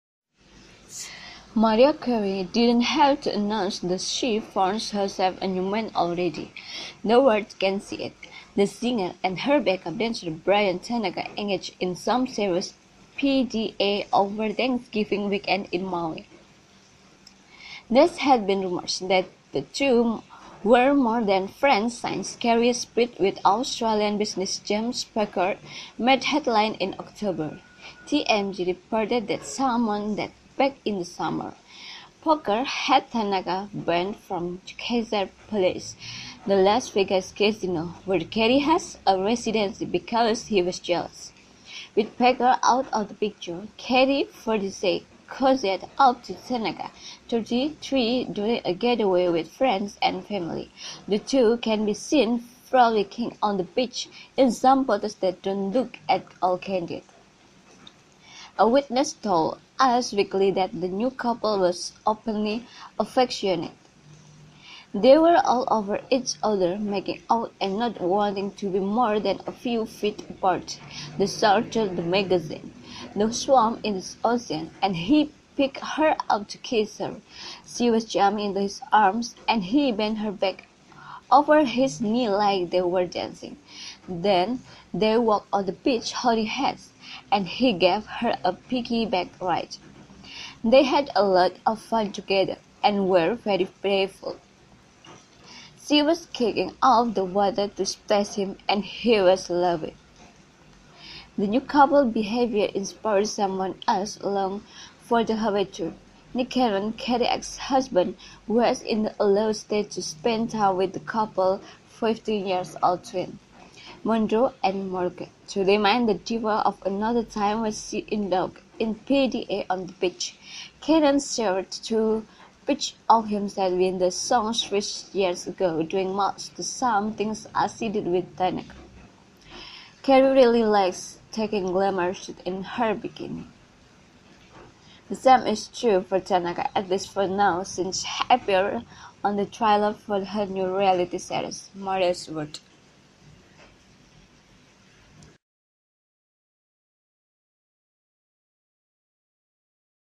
The New Romance of Mariah Carey and Bryan Tanaka in Hawai
Mariah Carey is a singer, writer and songwriter, record producer, and actress from the United States. Lately much was said about this one beautiful singer. Many are looking for news about her and her private life. After divorcing with her husband, many say that she had a special relationship with back up dancer, he is Bryan Tanaka. There are some photos of intimacy between Mariah Carey and Bryan Tanaka at the time they were playing on the beach in Hawai. The new couple’s behavior inspired someone else along for the Hawaii trip. This is an information about the new romance of Mariah Carey and Bryan Tanaka in Hawai, may be usefull.